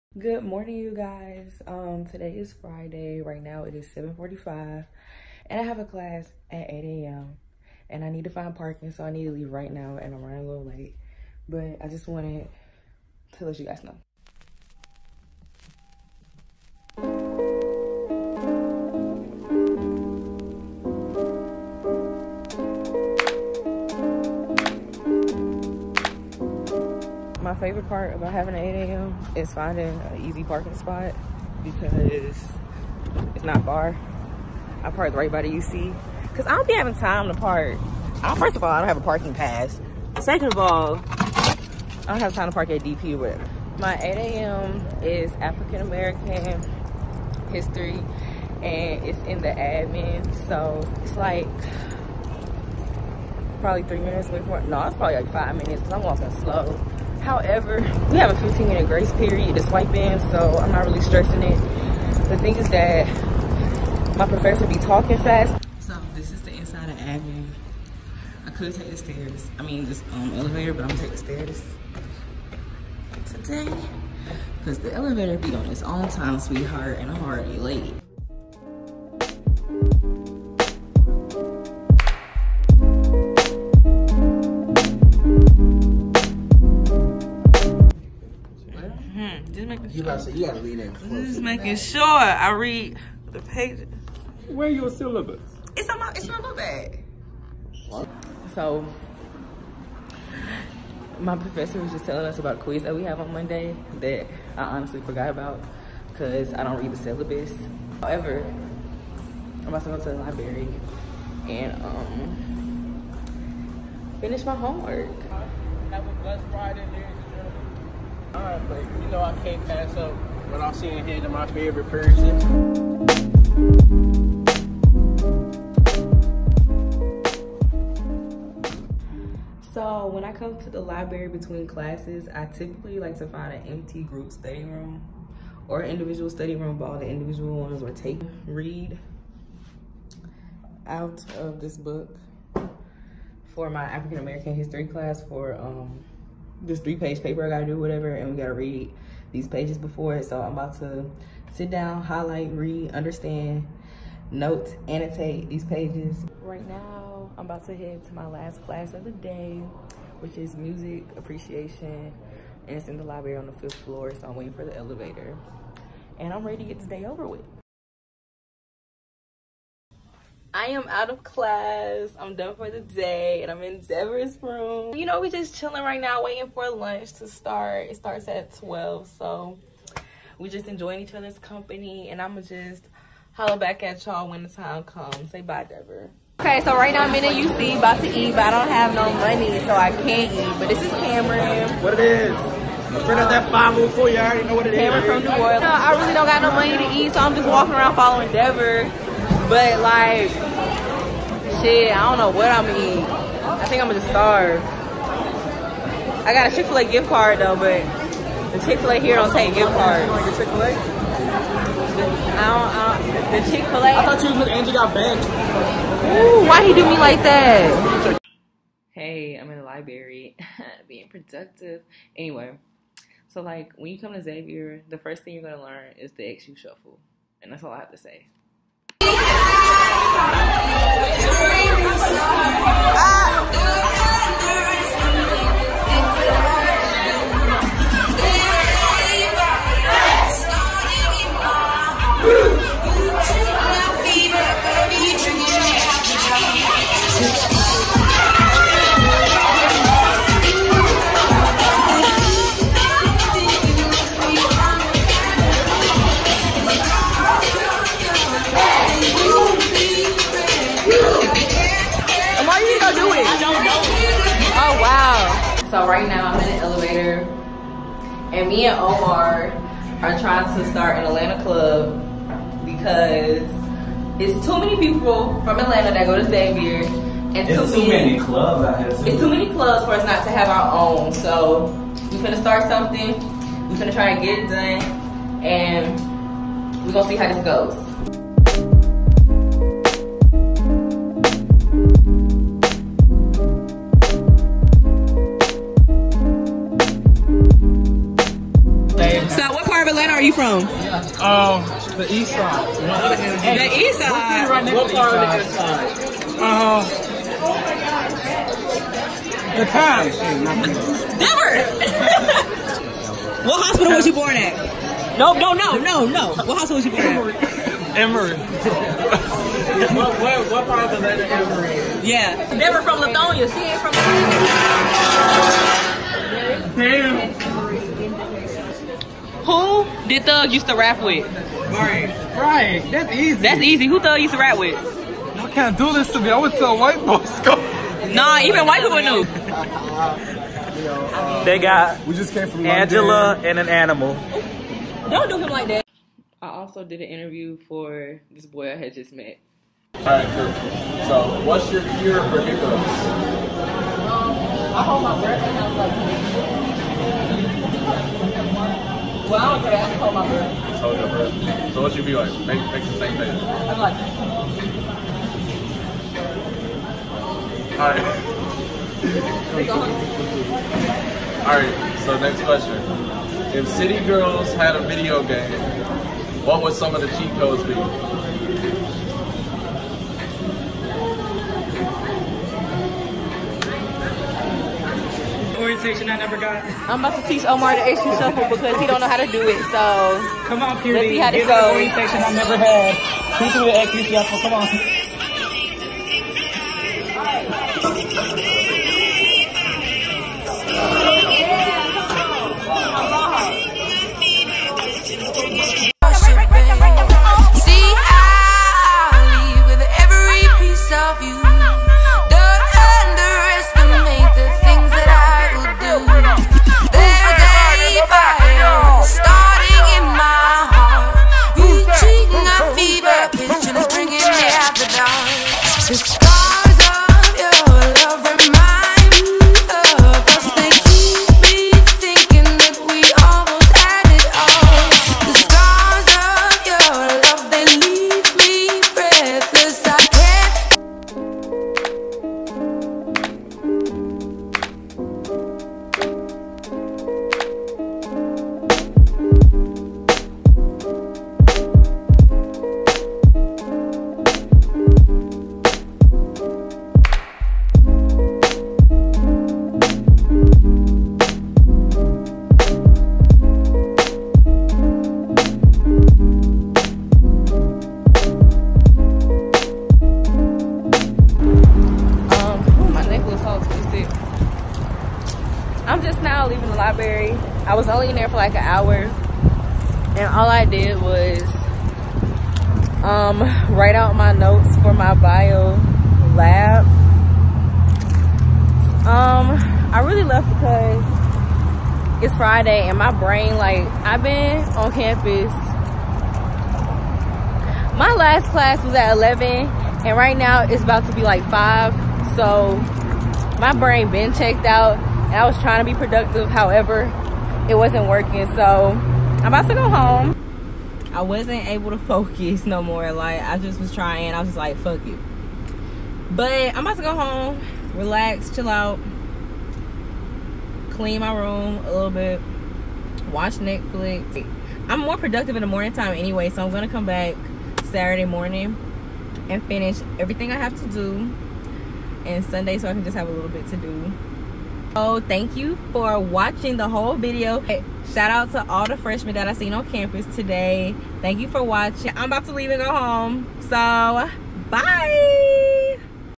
a day in the life of a hbcu college student @ xavier
so this is basically what I do on Fridays at school.
class, eat, joce & study!